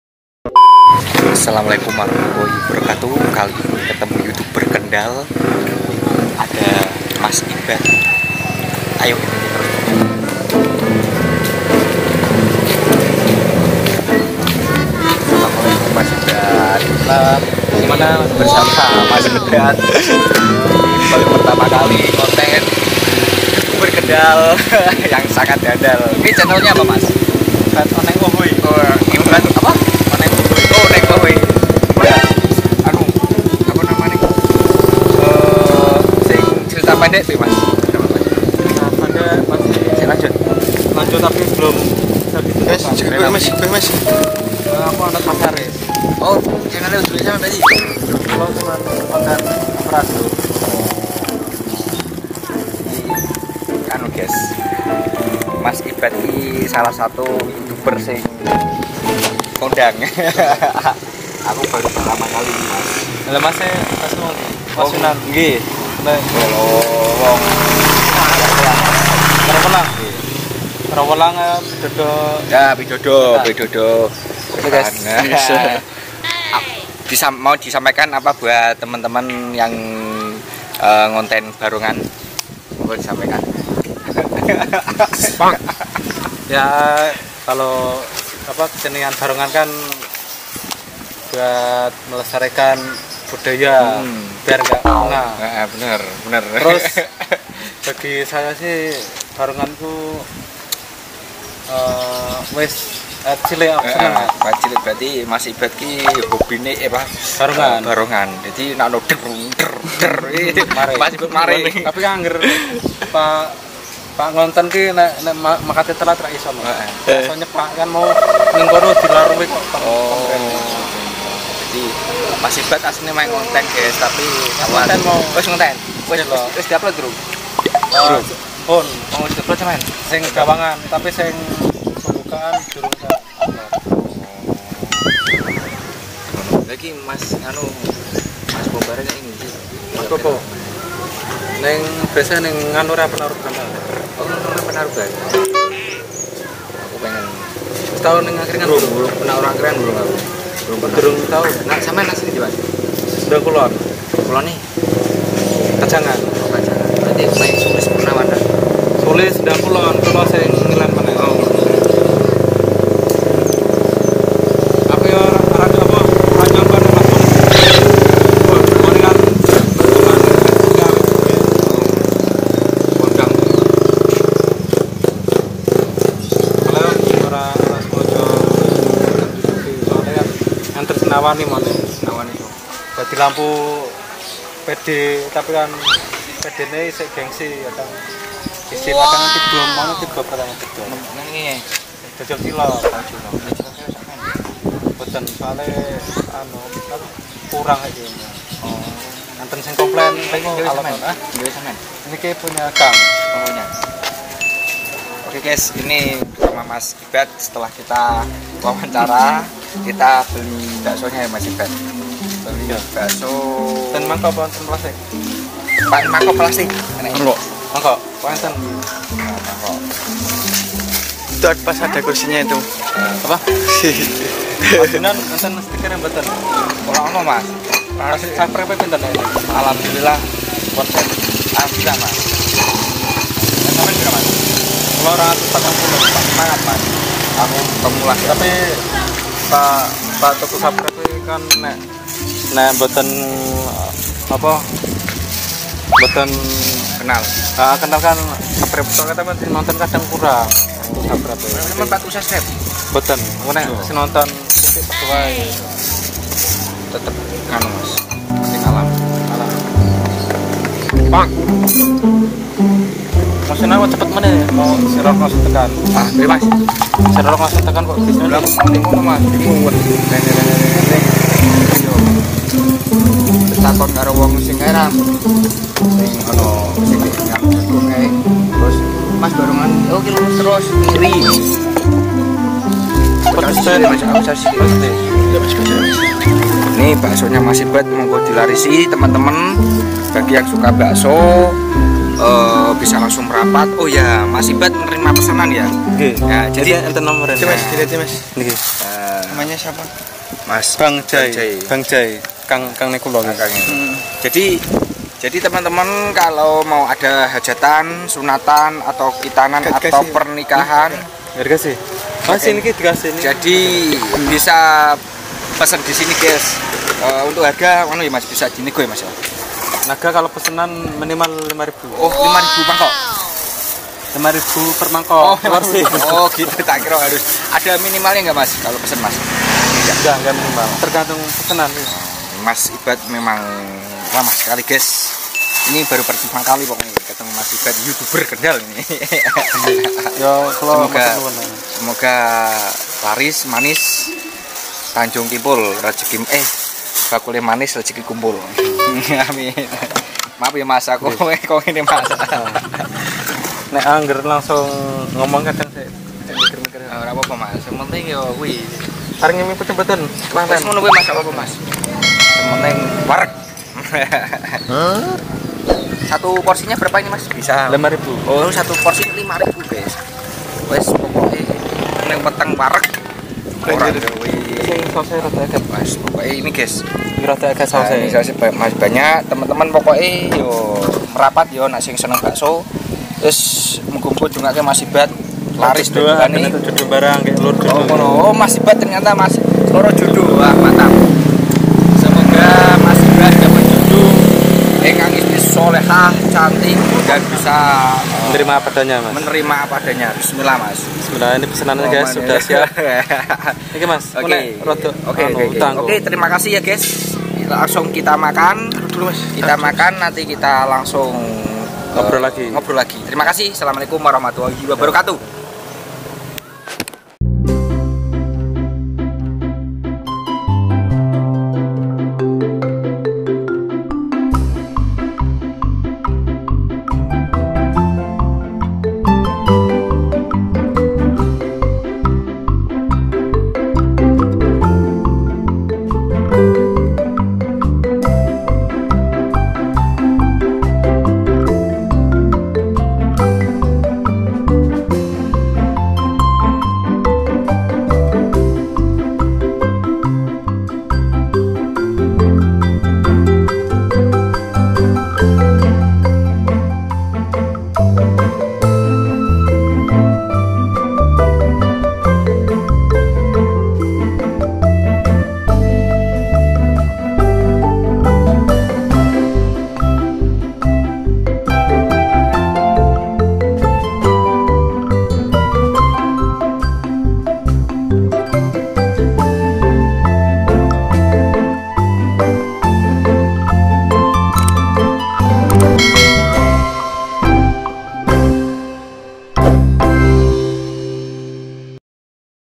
VLOG BAKSO WITH YOUTUBERS
Assalamualaikum kali ini ngonten bersama mas ibad vlog bakso with youtubers dari sendang dawung kendal jawa tengah.
Baru pertama ngevlog ternyata kang ibad orange unik dan ramah. Yuk larisi jualanya bang ibad baksonya sangat enak sekali gaessss.